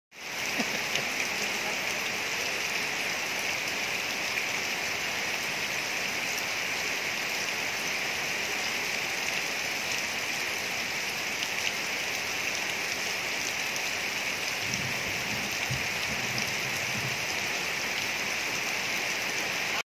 Caught in the rain
This channel contains a collection of natural sounds of thunderstorms packed with music, hopefully it can clear the brain and relax